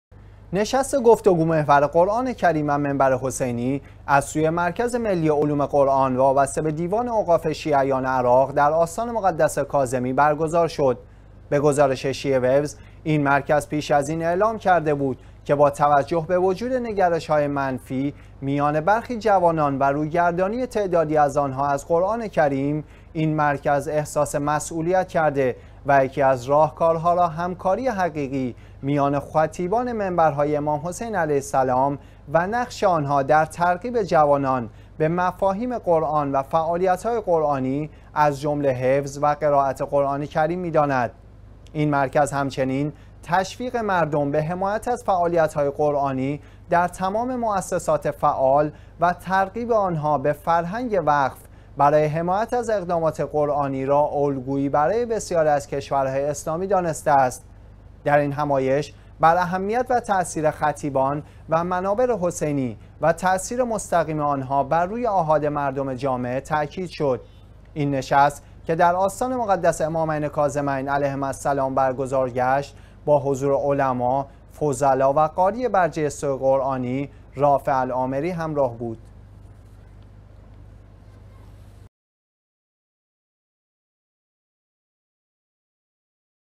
نشست «قرآن کریم و منبر حسینی» در آستان مقدس کاظمی
نشست گفت‌وگو محور «قرآن کریم و منبر حسینی» از سوی مرکز ملی علوم قرآن وابسته به دیوان اوقاف شیعیان عراق، در آستان مقدس کاظمی برگزار شد.
به گزارش شیعه ویوز، این مرکز پیش از این اعلام کرده بود که با توجه به وجود نگرش‌های منفی میان برخی جوانان و روی‌گردانی تعدادی از آنها از قرآن کریم، این مرکز احساس مسئولیت کرده و یکی از راهکارها را همکاری حقیقی میان خطیبان منبرهای امام حسین علیه السلام و نقش آنها در ترغیب جوانان به مفاهیم قرآن و فعالیت‌های قرآنی از جمله حفظ و قرائت قرآن کریم می‌داند.
 این مرکز همچنین تشویق مردم به حمایت از فعالیت‌های قرآنی در تمام مؤسسات فعال و ترغیب آنها به فرهنگ وقف برای حمایت از اقدامات قرآنی را الگویی برای بسیاری از کشورهای اسلامی دانسته است.
 در این همایش بر اهمیت و تاثیر خطیبان و منابر حسینی و تاثیر مستقیم آنها بر روی آحاد مردم جامعه تاکید شد.
این نشست که در آستان مقدس امامین کاظمین علیهما سلام برگزار گشت، با حضور علما، فضلا و قاری برجسته عراقی «رافع العامری» همراه بود.